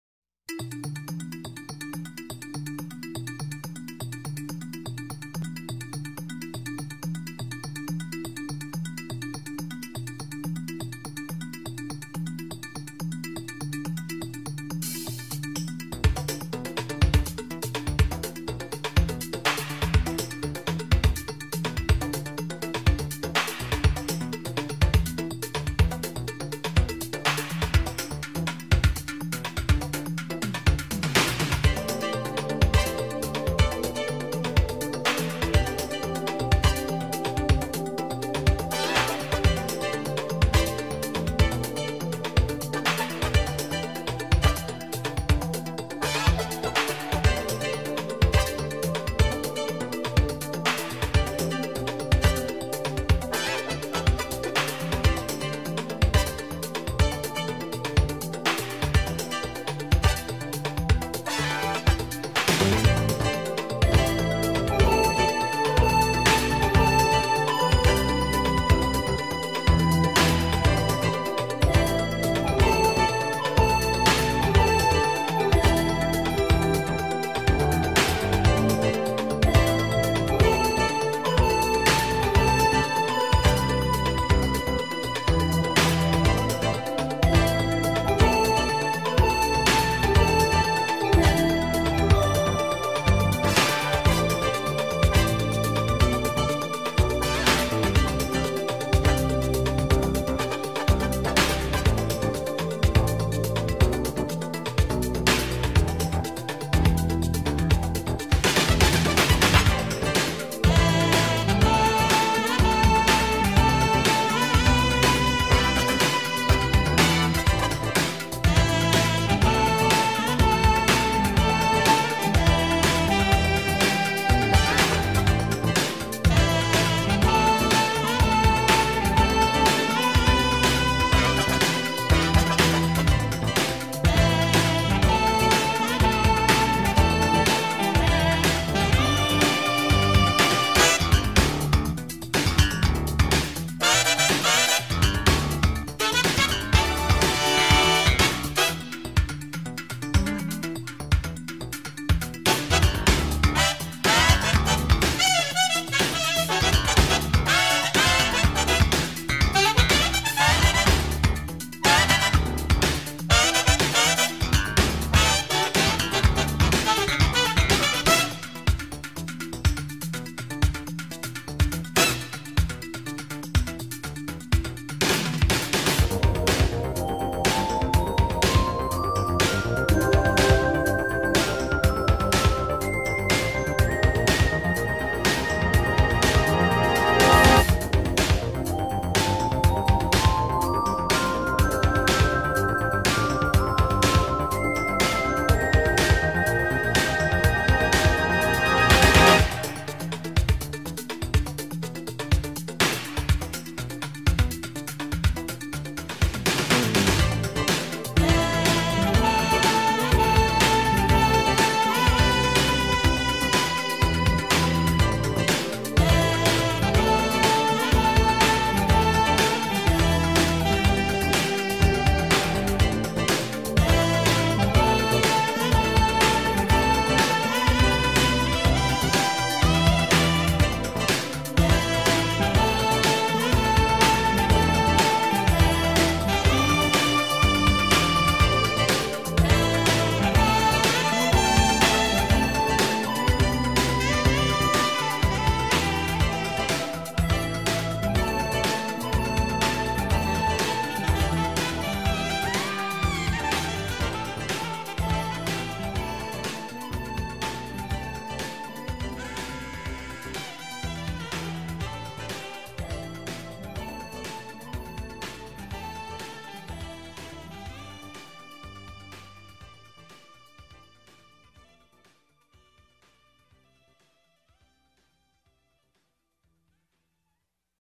The best of Downtown columbus 2019 HD
This video highlights the photos I taken this year of the downtown Columbus area